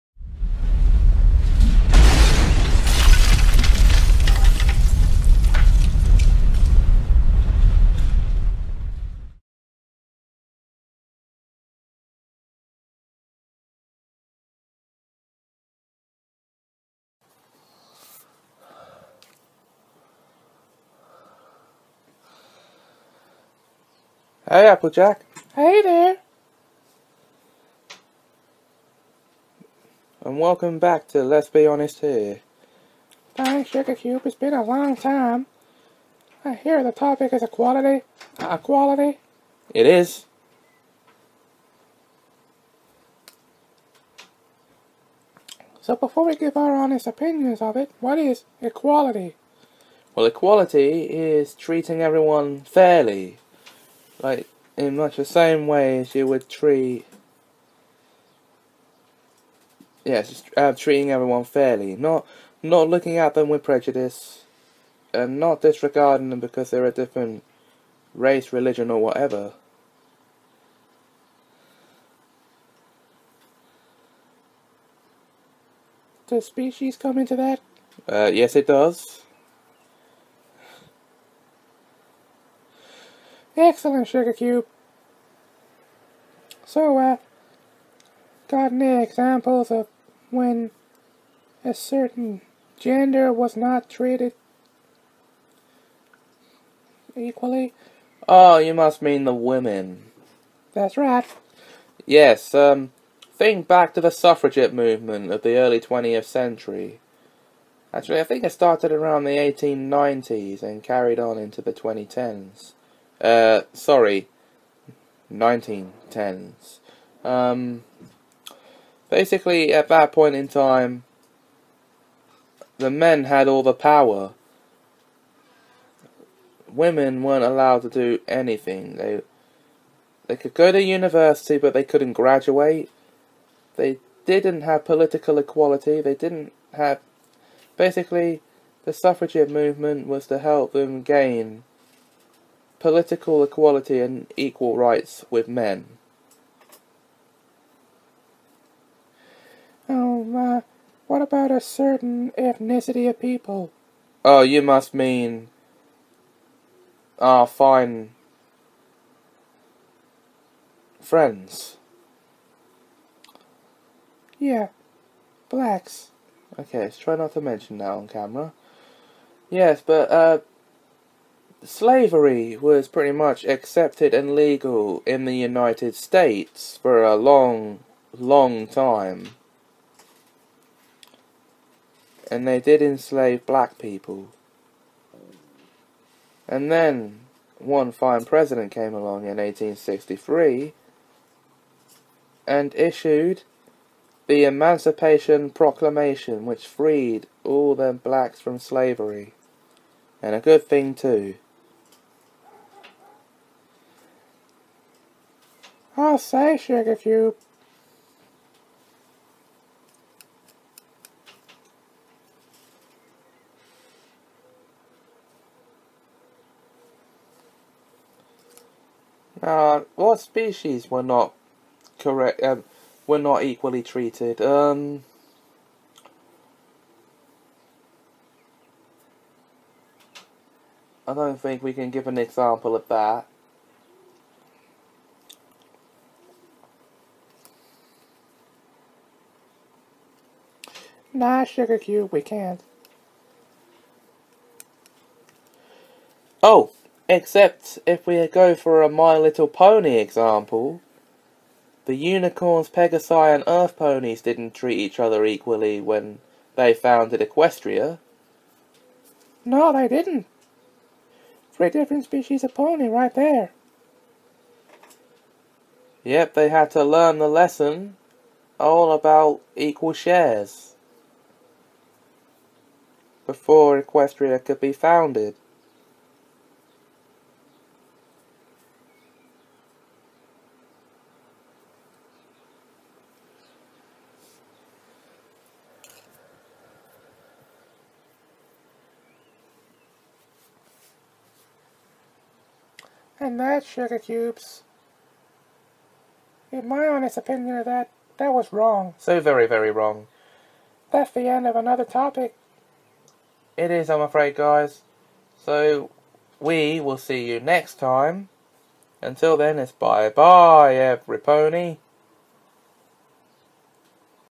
Episode 12: Equality
Me and Applejack discuss equality